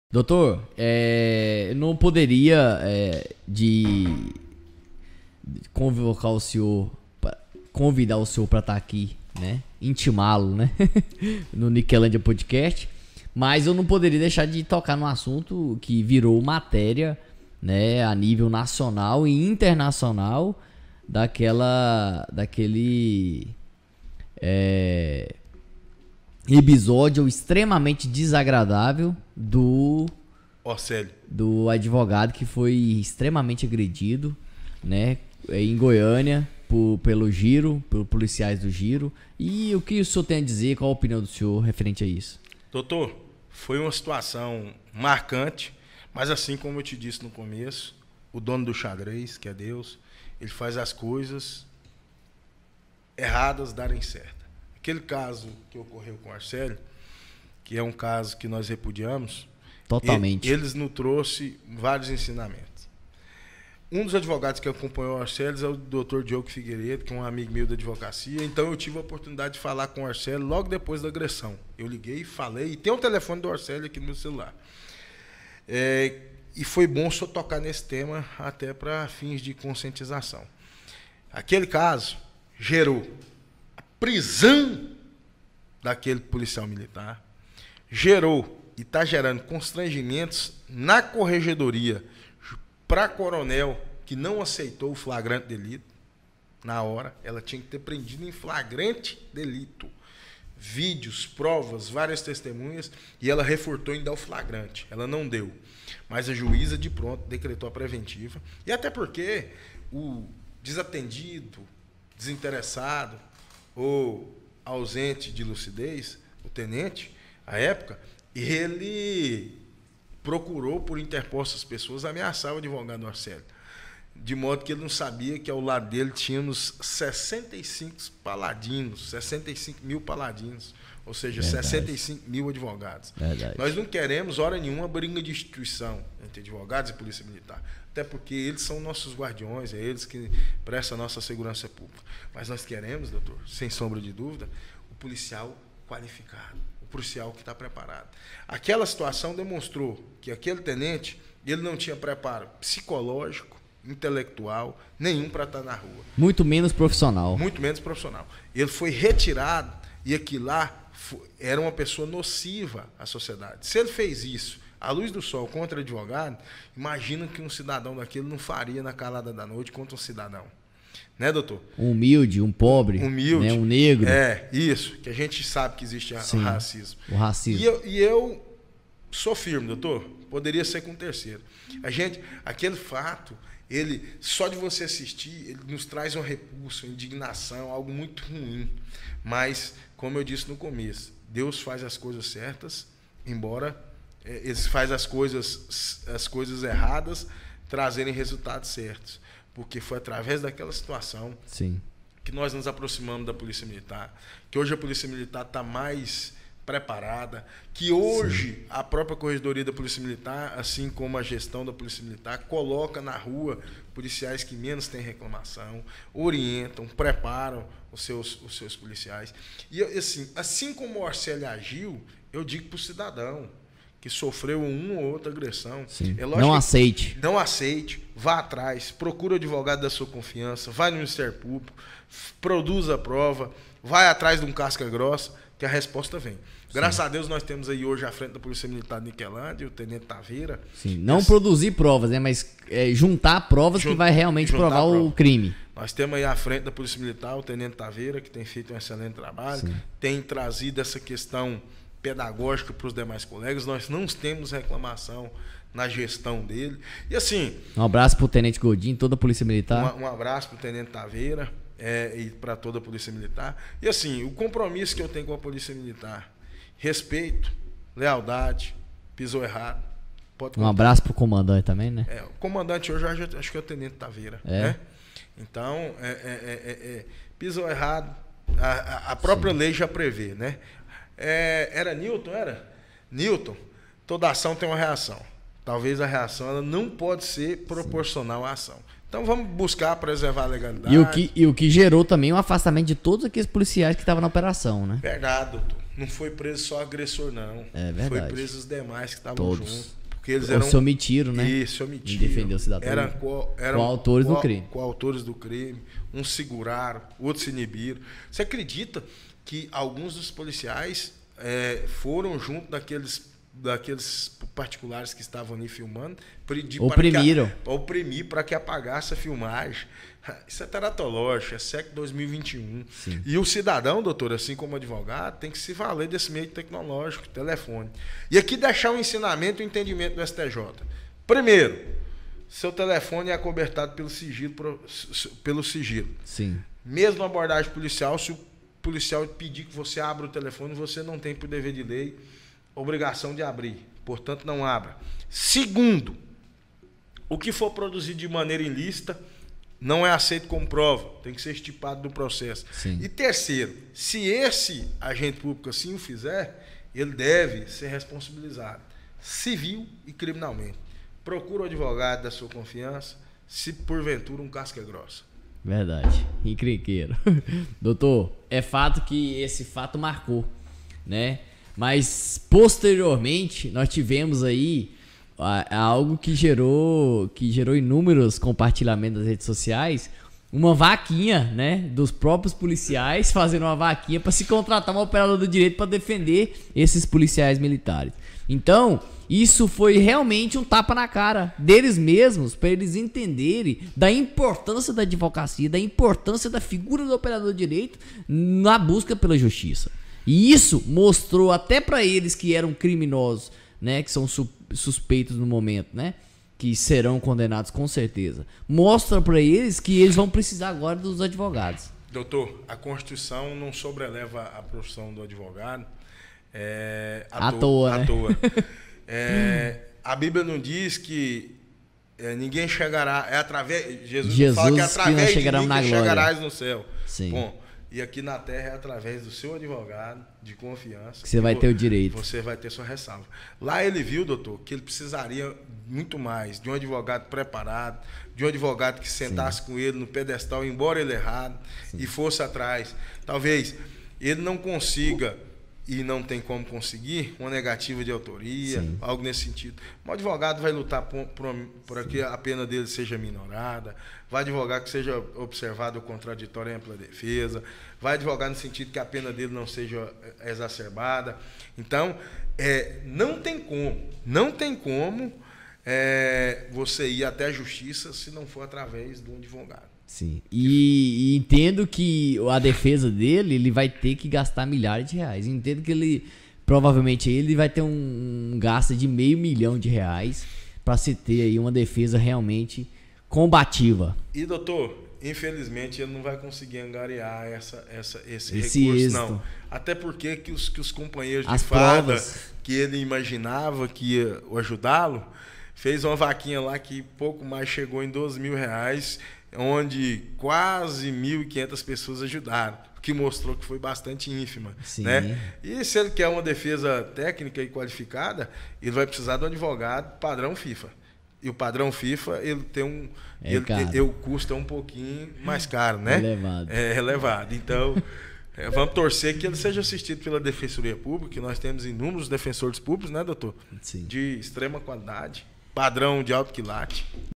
Niquelândia PODCAST, citamos e repudiamos o crime praticado contra a vítima Dr. Orcelio Ferreira.
Não poderíamos deixar de comentar as atrocidades que policiais militares praticaram contra o advogado Orcelio Ferreira Silverio Junior, de 32 anos, foi agredido pela Polícia Militar em Goiânia. Os vídeos foram matéria de todos jornais nacionais e internacionais, mostram o advogado sendo segurado por um dos agentes e levando socos e tapas. Ele chegou a ser arrastado pela calçada!